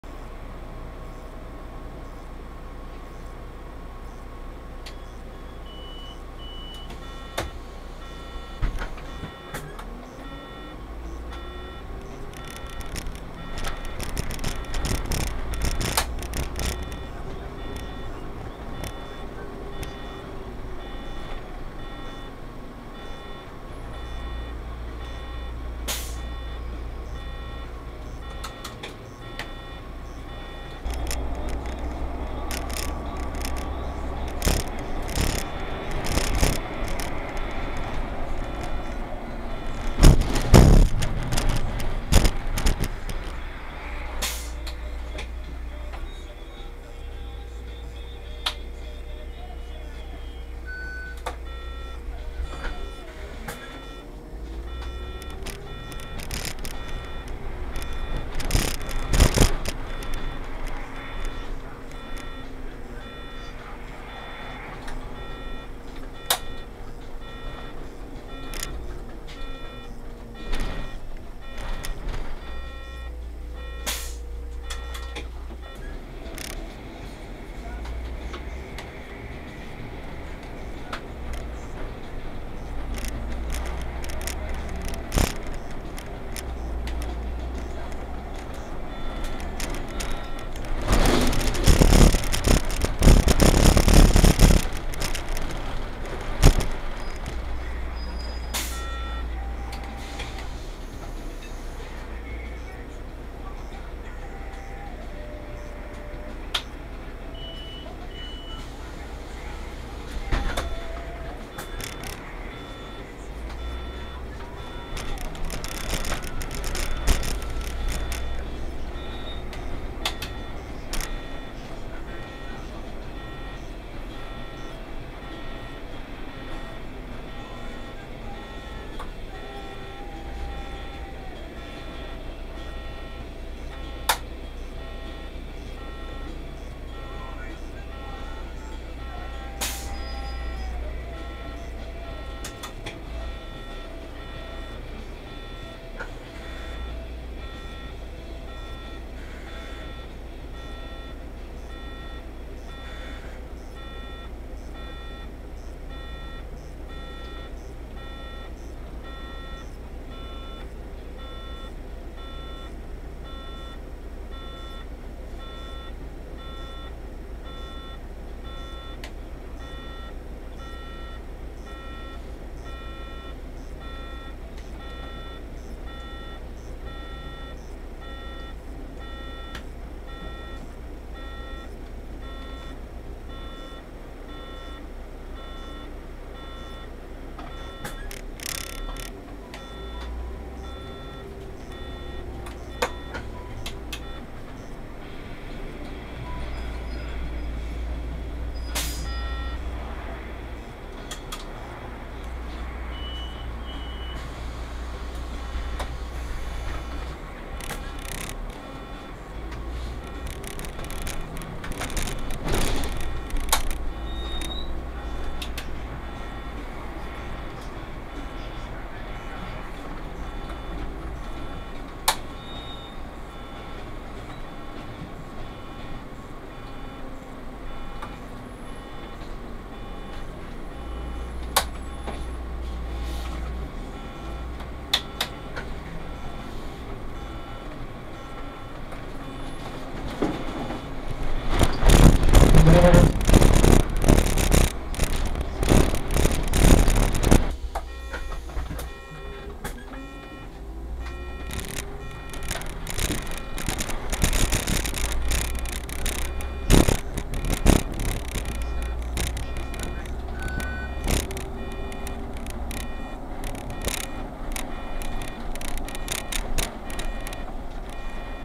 Dwa ciężkie etapy trasy na linii 136
Ulice: Żwirki i Wigury z Woronicza oraz Wołoska z Konstruktorską.

Na drugiej zmianie ponoć jest jeszcze gorzej.